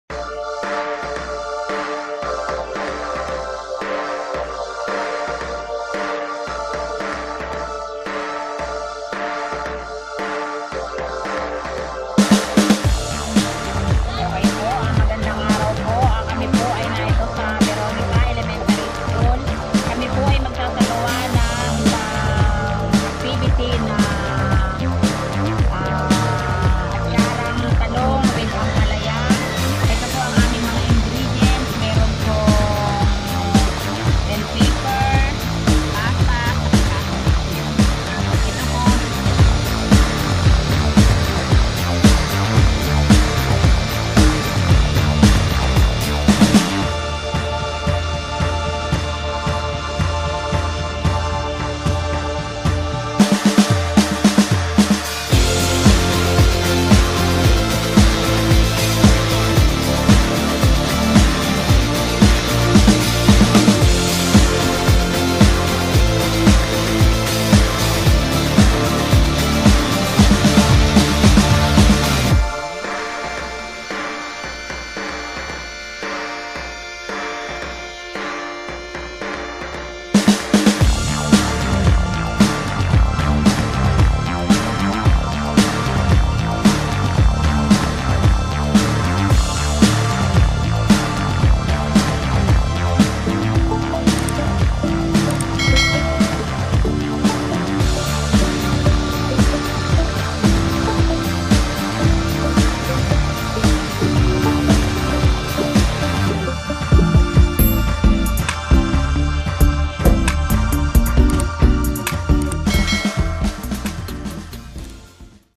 Income Generating Project (Atsarang Talong w/ Ampalaya)
IGP of Veronica Elementary  School
That is essential  and very economic  savings activity  both for  the students and parents of our school community.  This is not just an ordinary atsara that can buy from  the store,  it is nutritious  and easy to make.